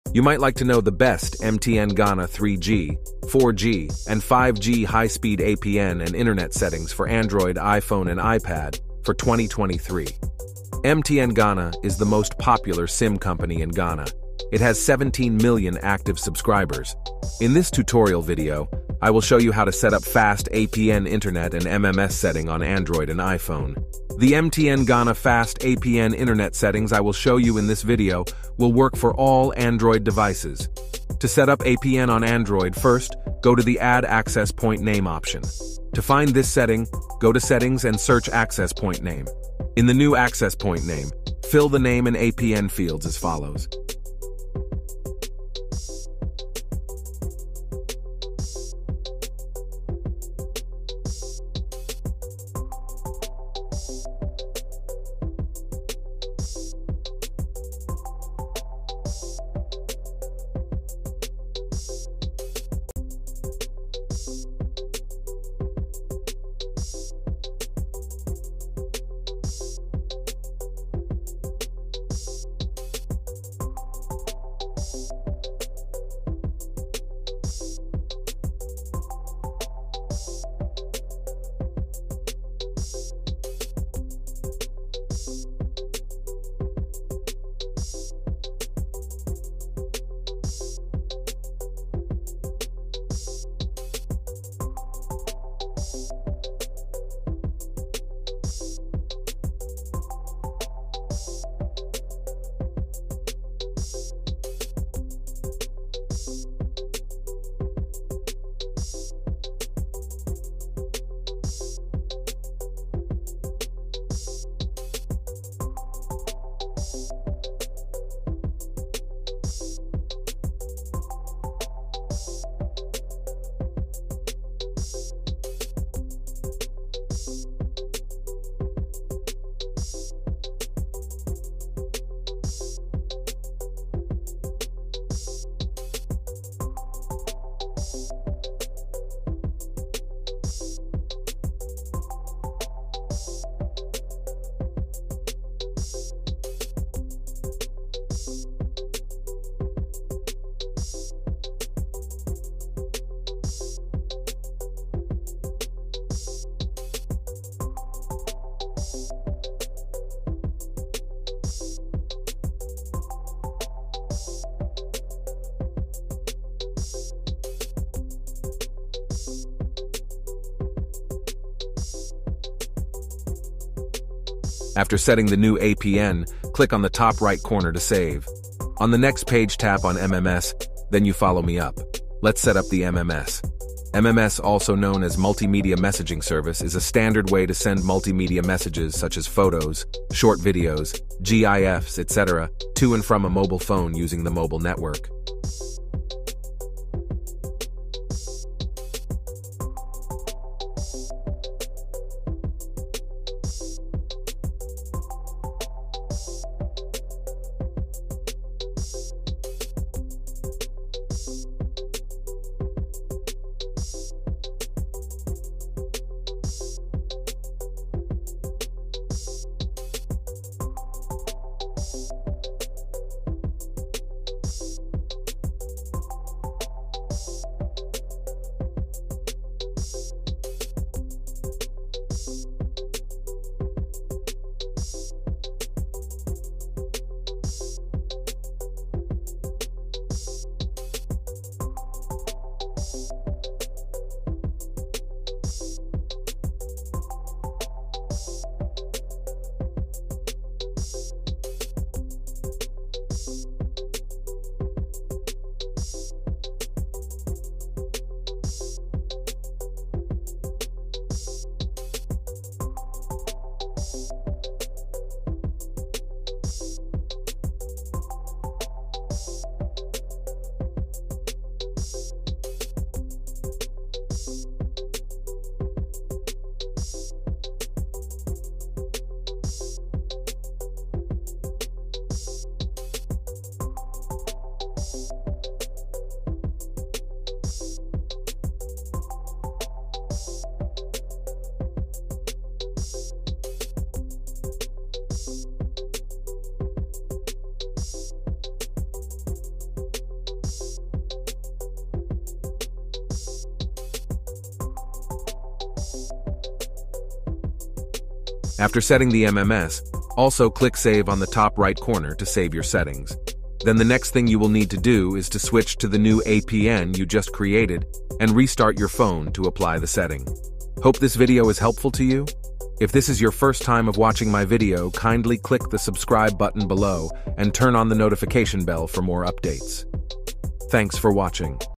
MTN Ghana APN MMS Settings for Android 2023
Are you looking for how to set MTN Ghana 2G 3G LTE 4G and 5G high-speed APN/ Internet settings for Android.

In this video, we will walk you through the process of setting up the MTN Ghana APN MMS settings for Android devices in 2023.

Having the correct APN (Access Point Name) and MMS (Multimedia Messaging Service) settings is crucial for seamless internet connectivity and the ability to send and receive multimedia messages on your Android smartphone. We understand that configuring these settings can sometimes be a bit tricky, especially when network configurations are updated.

To ensure you stay connected and enjoy all the features offered by MTN Ghana, we have created this step-by-step guide to assist you in setting up the APN MMS settings on your Android device. Our easy-to-follow instructions will help you navigate through the settings menu and configure the necessary options correctly.

Our team is here to assist you and make sure you have a smooth and enjoyable mobile experience.

Thank you for choosing our channel for your tech needs. Let's get started with the MTN Ghana APN MMS settings configuration on your Android device!